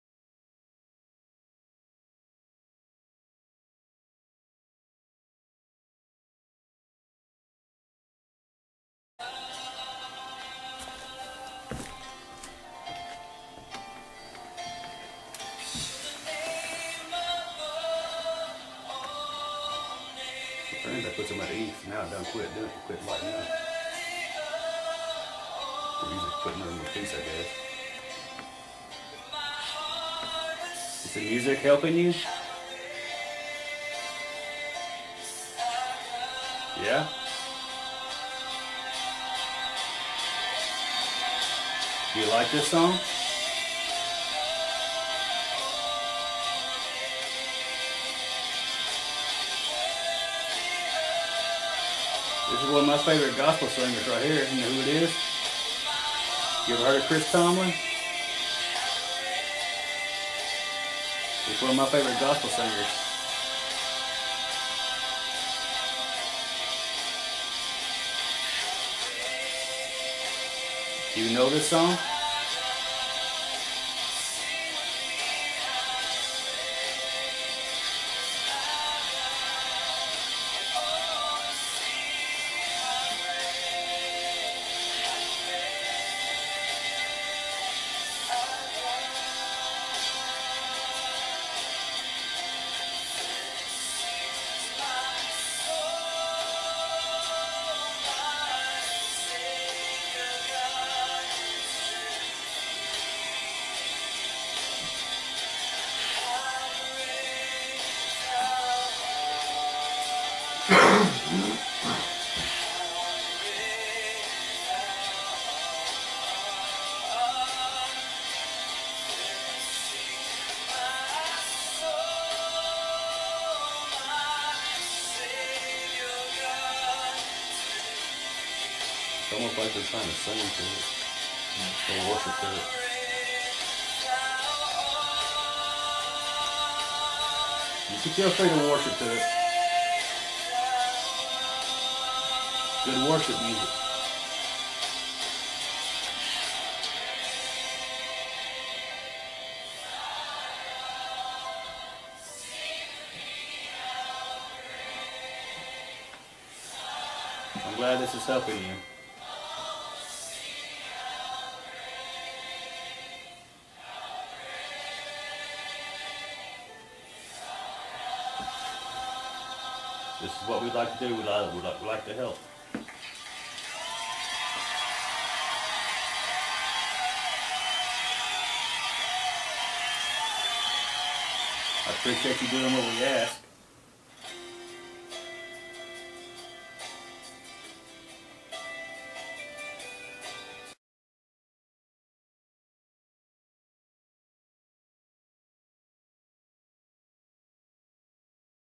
K2 Responding To The Music
This was captured at an old Princeton house. We decided to try a little experiment. We'd heard the family who once lived in the house were really religious. So we decided to play some Gospel music to see if we'd get a response. We placed the K2 Meter on the table across from us and it started reacting to the music and our voices.